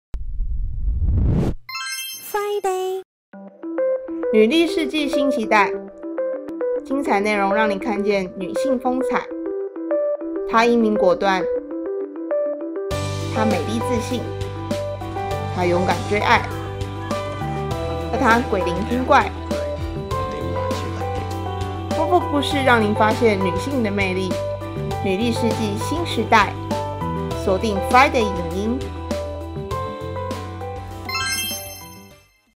friDay影音｜2015 年度特輯｜女力世紀 新時代
2015 年女性新力量的崛起！從政治、經濟、文化到社會關懷，女性為世界帶來不一樣的思維！friDay影音 特別策劃「女力特輯」，與你從電影、戲劇、新聞等多元的角度見證這個時代的 Herstory。【立即前往活動網頁領取免費看影展序號】